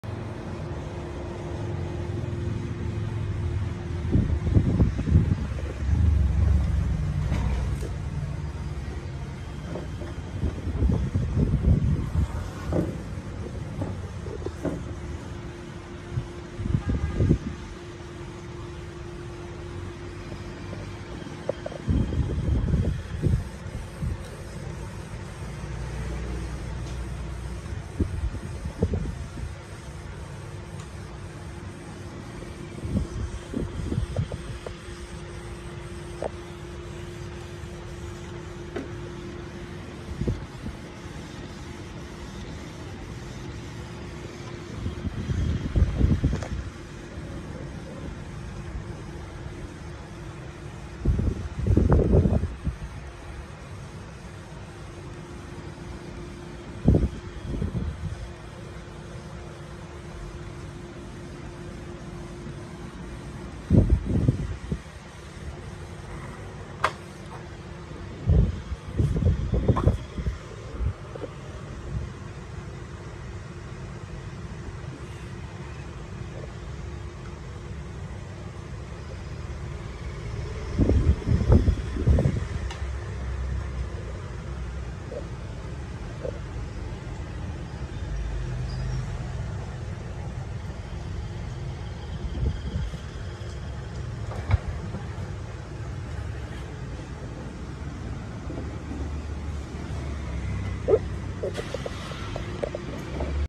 Test ảnh mực xịn bền màu chuẩn màu dành cho máy canon ix6830-ix6770-ix6870 giấy lụa giấy lụa-bóng
Call Zalo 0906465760 tư vấn mực và máy